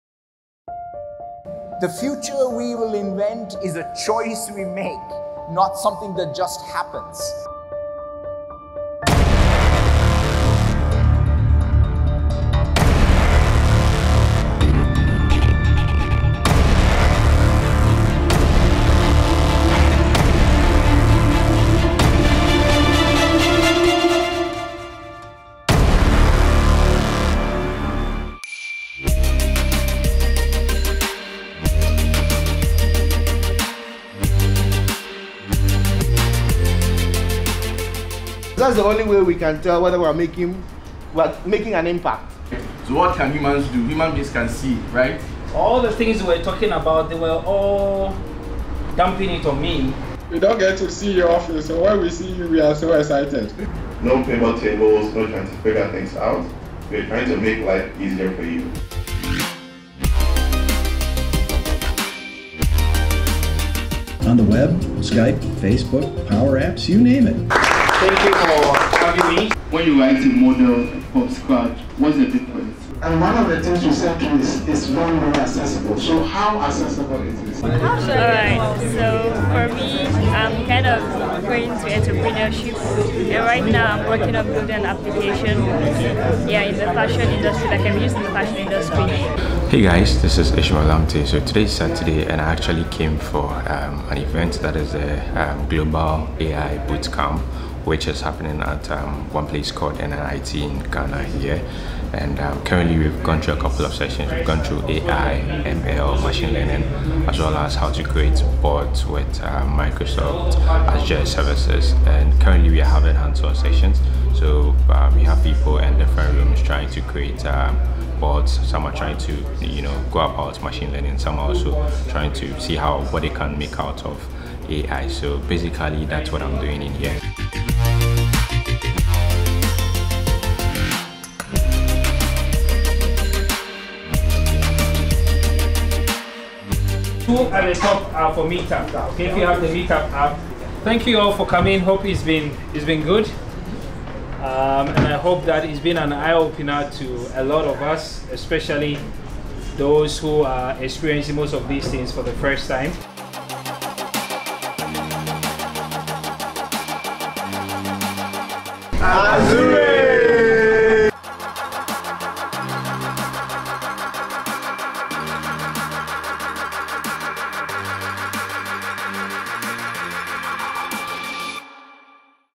Global AI (Artificial Intelligence) Bootcamp 2019 - Accra, Ghana
The Global AI Bootcamp was a free one-day event organized across the world by local communities that are passionate about artificial intelligence, Machine Learning, and creation of Bots on Microsoft Azure services.

The event took place on December 14th, 2019 with venues on every continent, The Accra Community held theirs at the NIIT institute in Accra and it was hosted by the by Accra .Net User Group.

The Global AI Bootcamp is centrally managed by our HQ in the Netherlands with support from Microsoft. The event is a perfect balance of quality content, awesome talks, and hands-on learning with like-minded peers in your community.